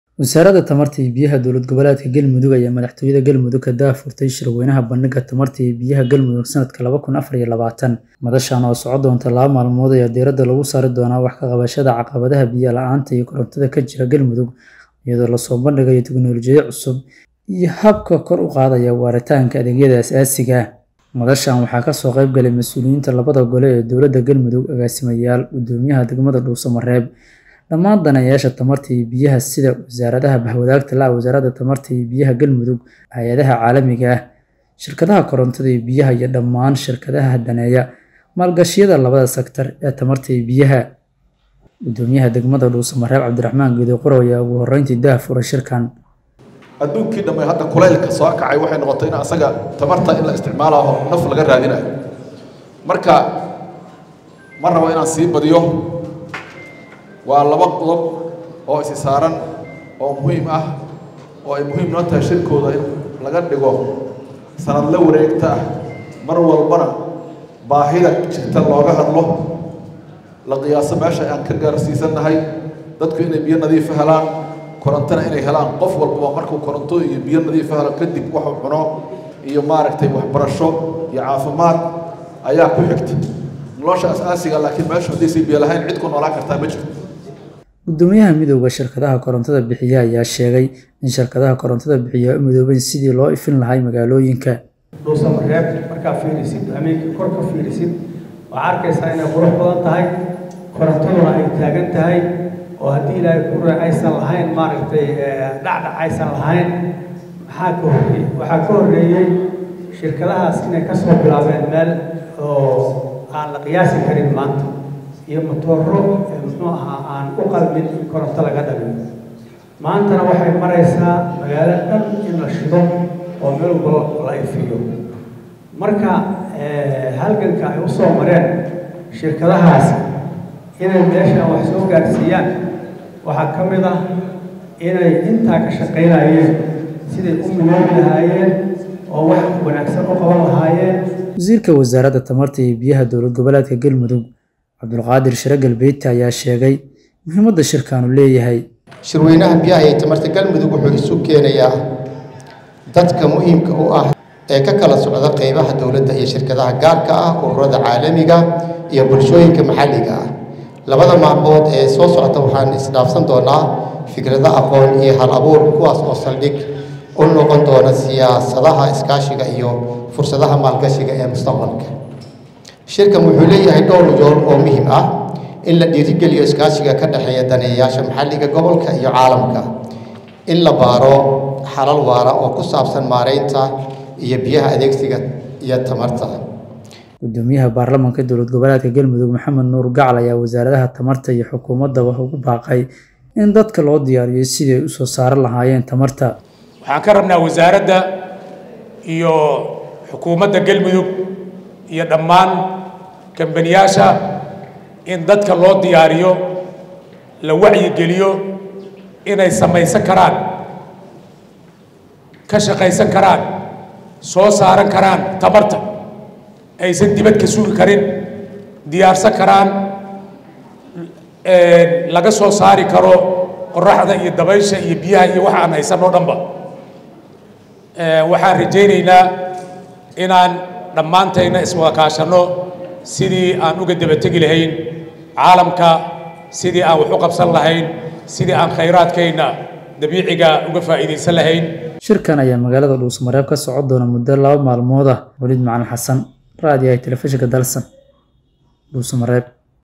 Wasaaradda Tamarta iyo Biyaha Galmudug Dhuusamareeb ka daah furtay Shorweynaha Bandhigga TamartA
Wasaaradda Tamarta iyo Biyaha Galmudug Dhuusamareeb ka daah furtay Shorweynaha Bandhigga Tamarta Sanadka 2024.